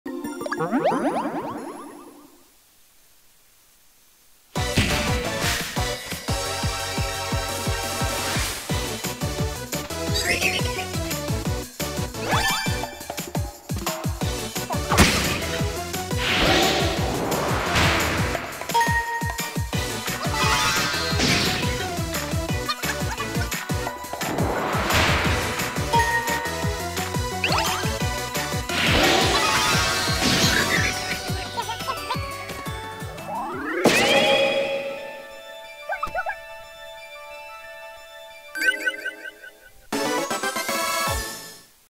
Butterfly "Carefree Guide" 1 turn KO
Both party members are at level 2.
Just wanted to show off a very lucky activation of the Stubborn Mii's "Again".
By the by, the Mage Mii is Nine the Phantom from Blazblue